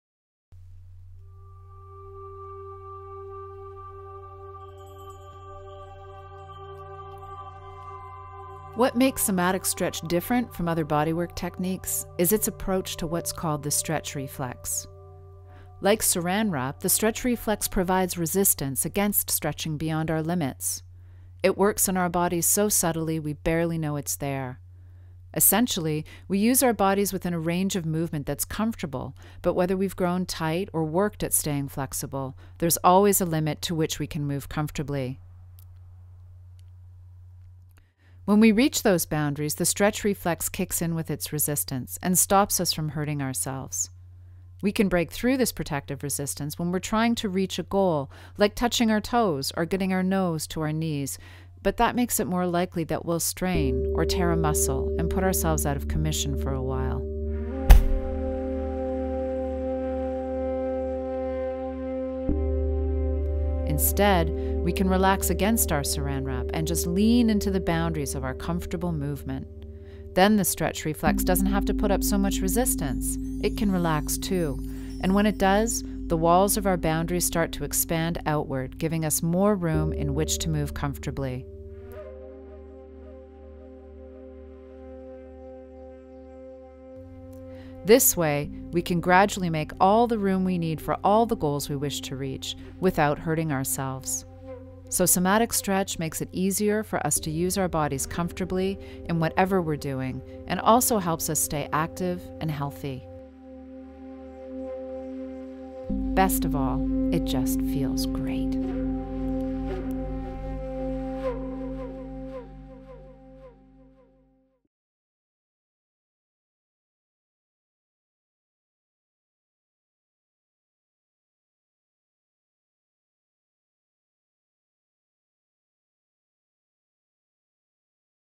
The Stretch Reflex with Somatic Stretch®
This 2-minute animation describes the Stretch Reflex and how and why we use it in Somatic Stretch®!
Sign up for free videos on our website.